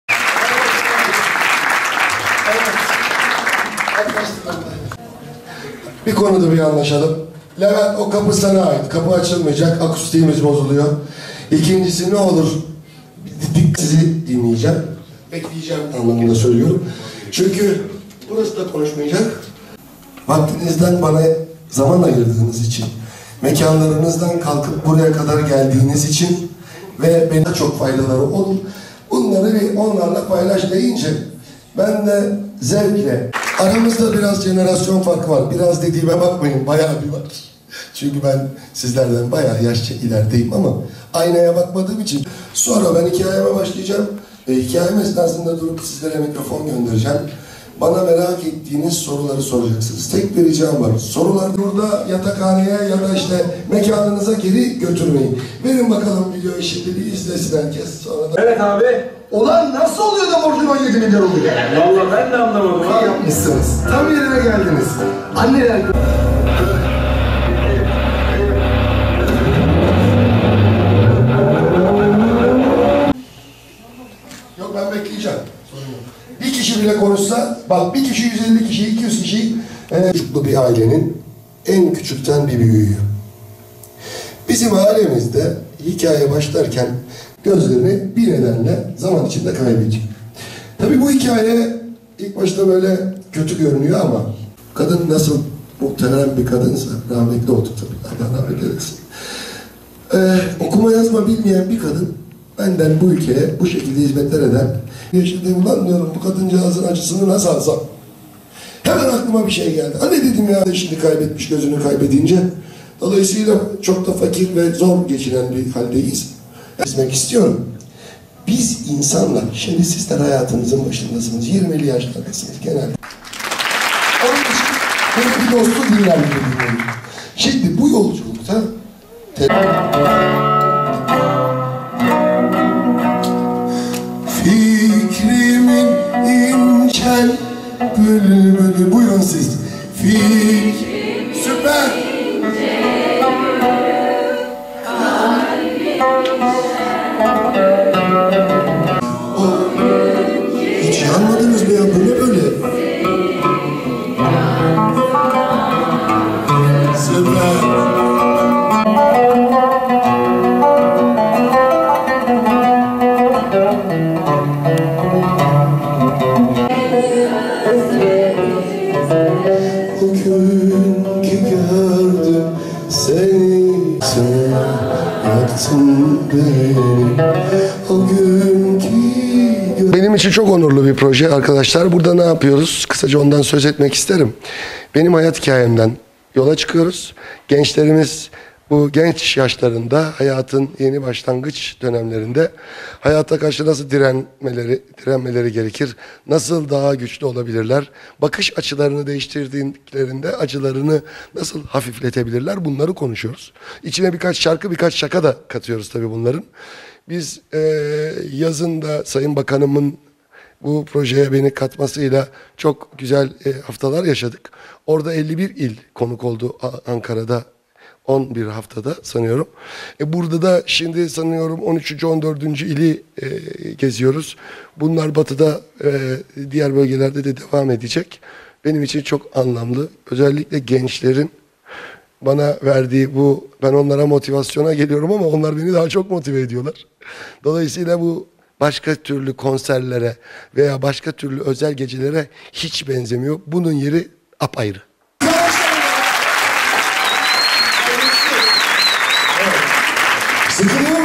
Metin Şentürk, üniversite öğrencileriyle buluştu
Ünlü sanatçı Metin Şentürk, Elazığ’da KYK yurtlarında kalan üniversite öğrencilerini hem eğlendirdi hem de hayat tecrübelerini paylaştı.